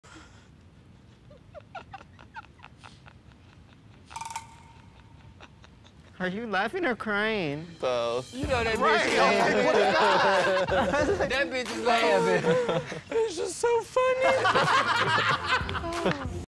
Willow Pill Confusing A Whole Community For 16 Seconds
Willow pill laughing and crying at the same time